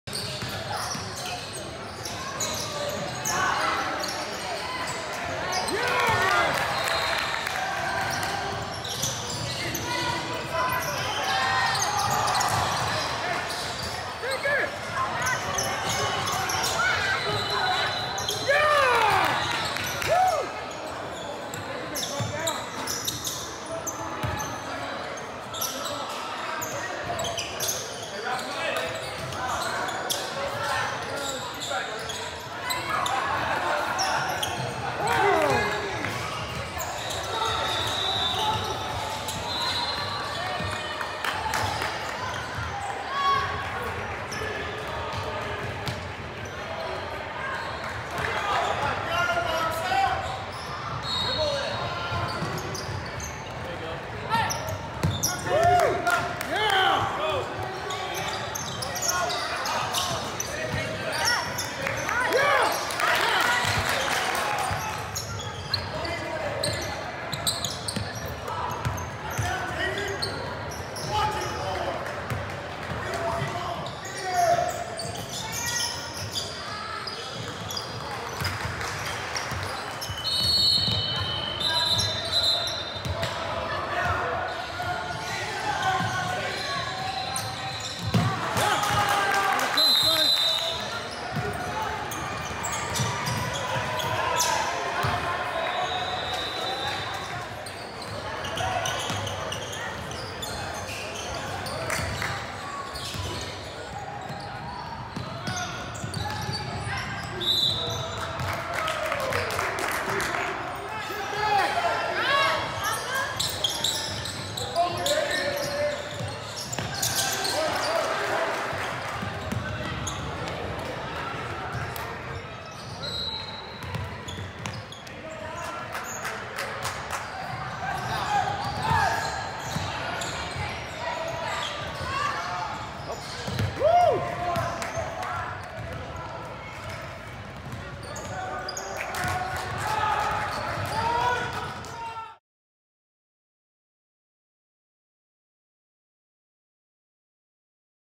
DTermined Athletics “Spring Classic” Basketball Tournament -Open Gym Premier - Max Camacho 5th Grade
Player - Max Camacho
Age - 11 years old
Grade - 5th
Graduating - Class of 2029
Position - Center
Team - Open Gym Premier OGP 11U
Tournament - DTermined Athletics Spring Classic
Date - April 2022
Facility - Get It Done Sports Arena
Location - Corona, California